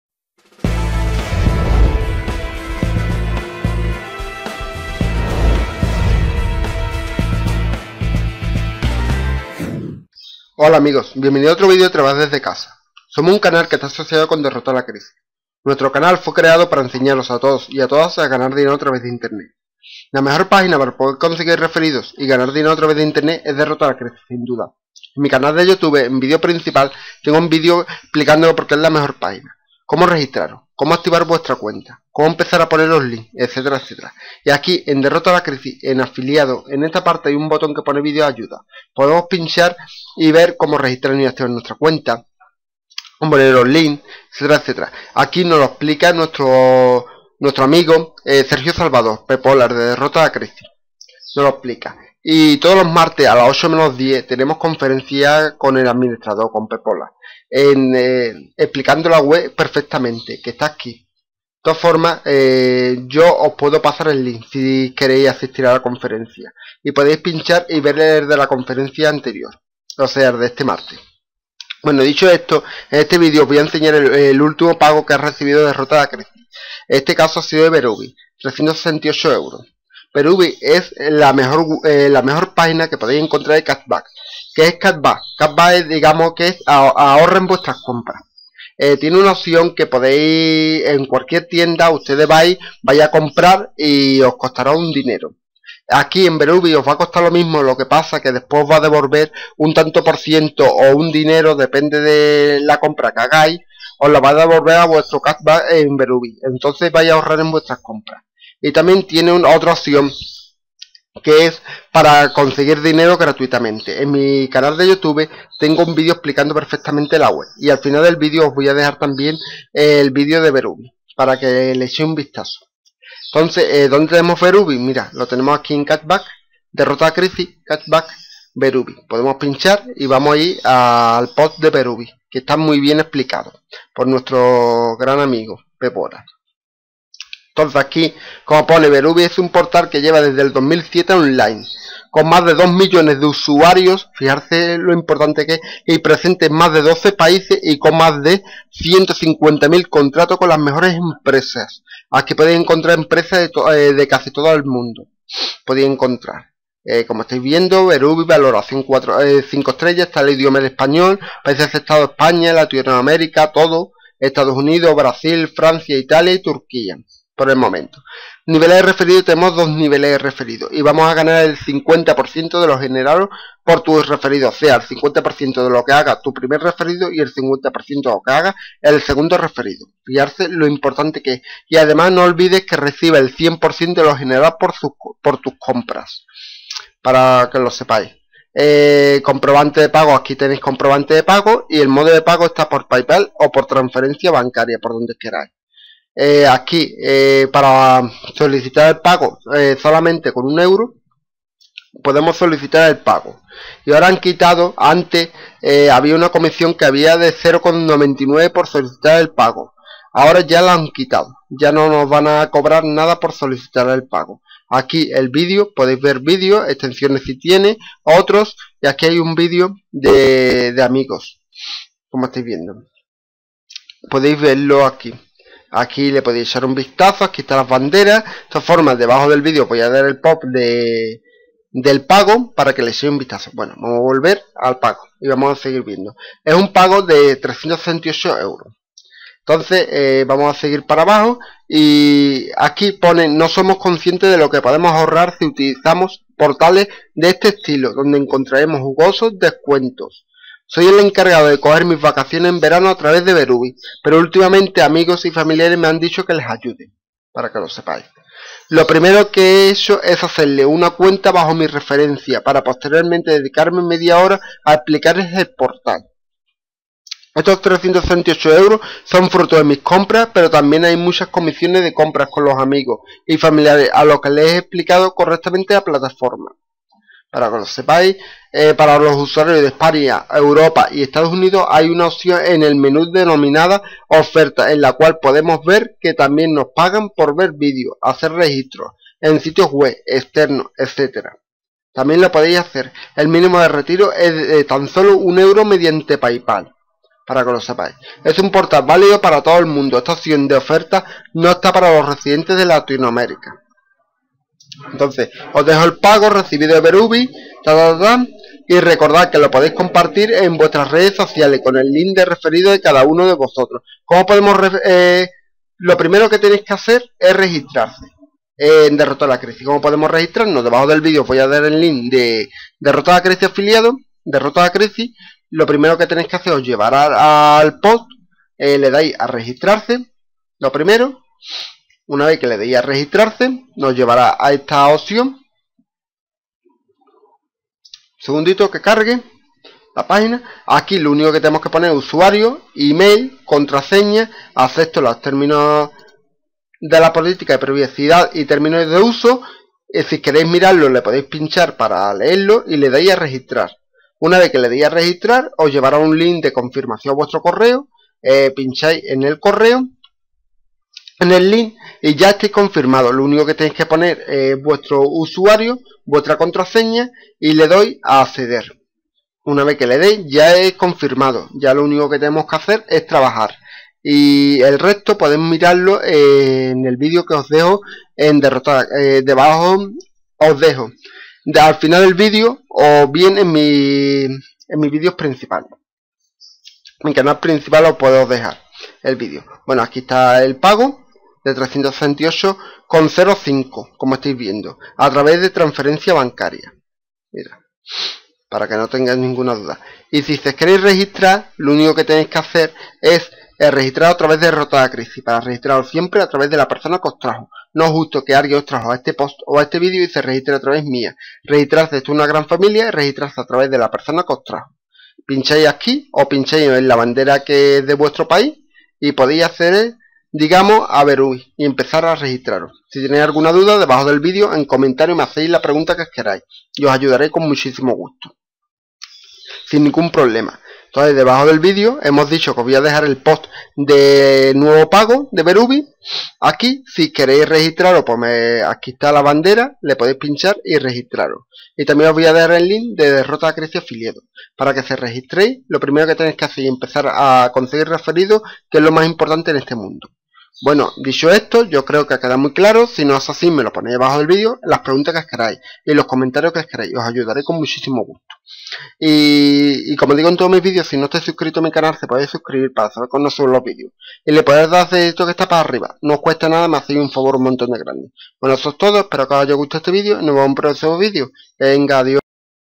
Beruby, Nuevo Pago 368€ por Transferencia Bancaria | Mejor Pagina CashBack Mundo | Derrota la Crisis
Hola amigos, en el vídeo de hoy os enseño el ultimo pago de 368€ en Beruby, la Mejor Pagina de CashBack (Ahorra en tus Compras) del Mundo en DerrotalaCrisis a través de TransferenciaBancaria.

Beruby es una Pagina valida para Todo El Mundo en el cual pueden Ahorrar en sus Compras y también tiene un apartado para GanarDinero Gratis.

La verdad que tenemos que ser conscientes lo que podemos Ahorrar en nuestras Compras con portales como este.

Para los usuarios de España, Europa y EstadosUnidos hay una opción en el menú denominada Ofertas en la cual podemos GanarDinero Gratis ver Vídeos, HacerRegistros, etc.

No podéis perder esta oportunidad ya que de verdad vais Ahorrar bastante en vuestras compras como podéis ver en el vídeo.

Bueno amigos, me despido de ustedes hasta próximo vídeo.